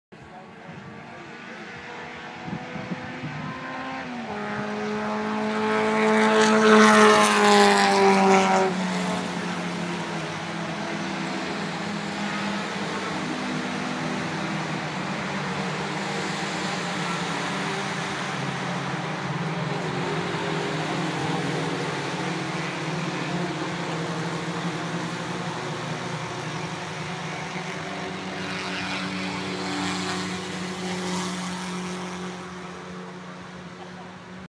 Pitt Races Beaver Run July 14 2012 Group 1 #1
Group 1 drivers Chris Shoemaker, Christian Morici.

For the list of all the racers go to this web all

This group is Open wheel action with Formula Ford and Club Ford, Rear engine Formula Juniors, Formula B Formula 2 and LeMans Style Prototypes.